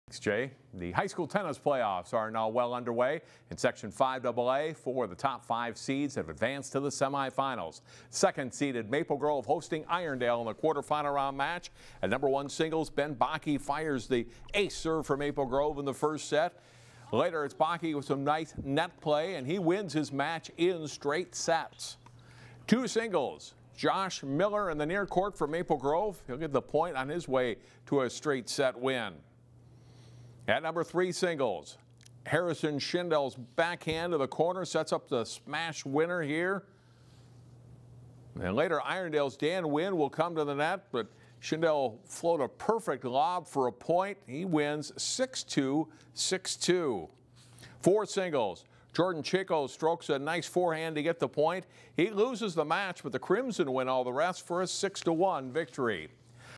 Maple Grove Advances in 5AA Boys Tennis
The Maple Grove boys tennis team defeated Irondale 6-1 Wednesday to advance to Thursday’s semifinals of the Section 5AA tournament. The Crimson meet Spring Lake Park in the semifinals with the section final scheduled for next Tuesday.